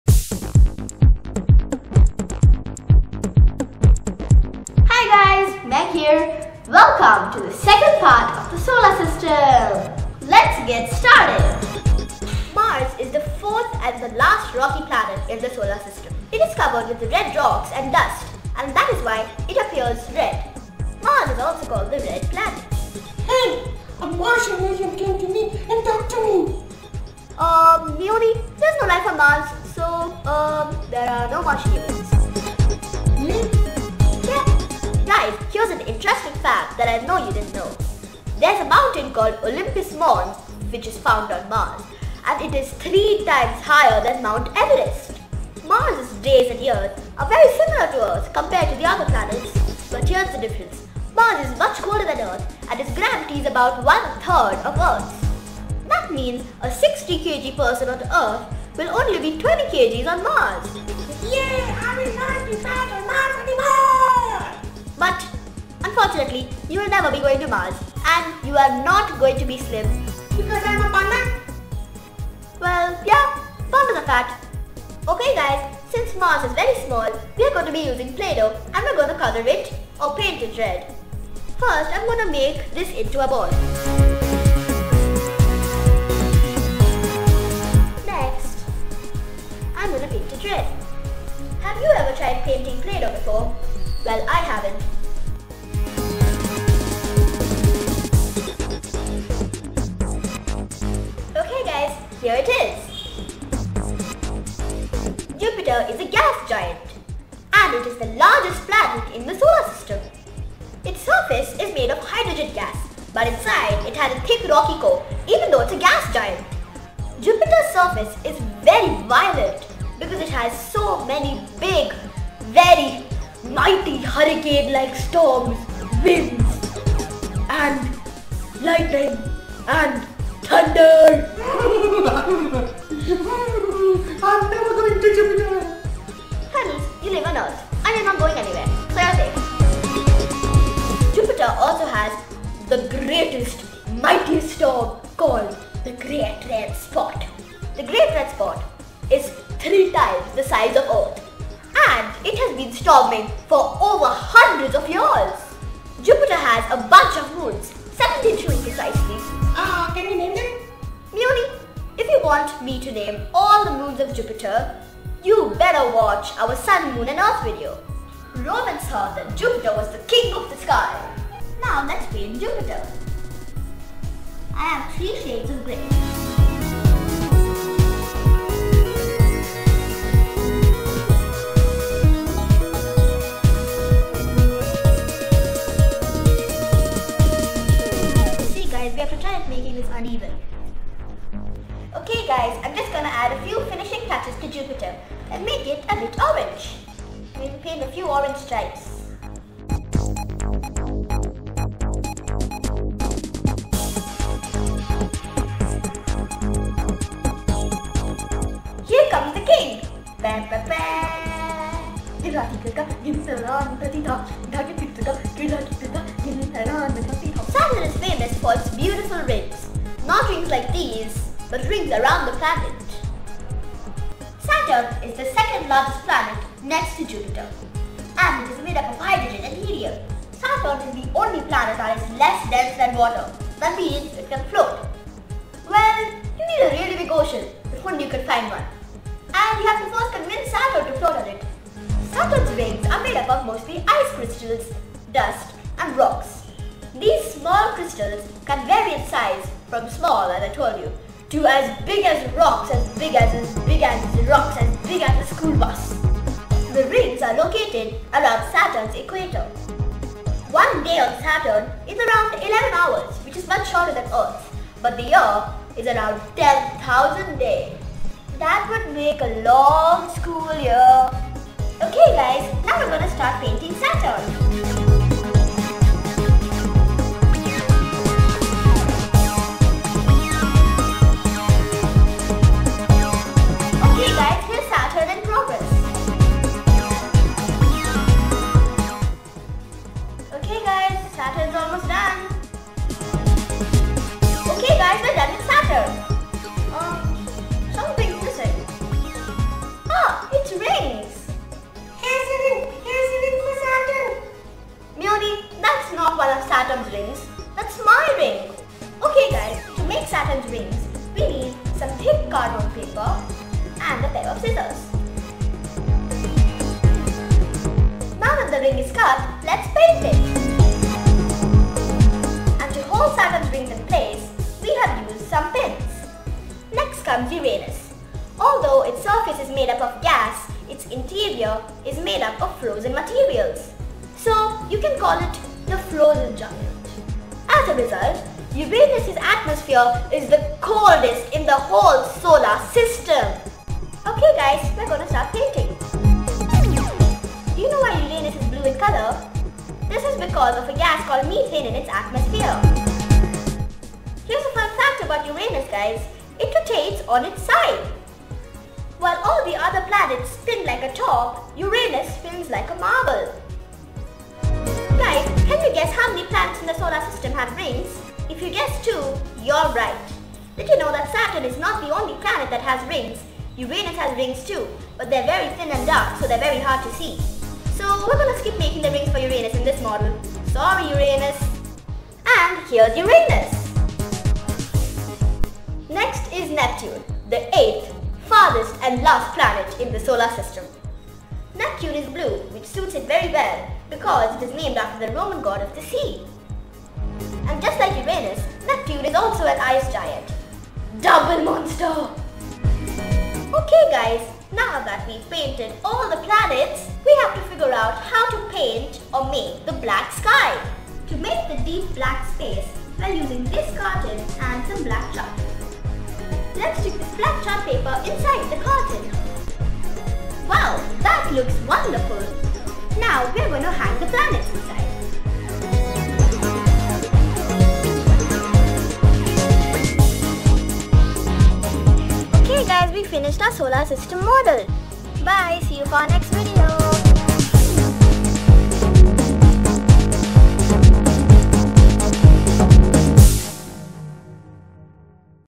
Solar System for kids : facts, fun and craft : Making a model - part 2
Fun filled educational video for children about solar system. Make a model of solar system with Meg and friends. palsnpupils brings another science videos for kids stemkids sciencekids solarsystem Nursery and pre-primary school kids will enjoy this hilarious video with lot of learning while having fun making a dazzling model of our solar system. Learn about Mars, Jupiter, Saturn, Uranus and Netune in this educational and fun video. Did you know that Olympus Mons is 3 times taller than mount everest - which planet is it on?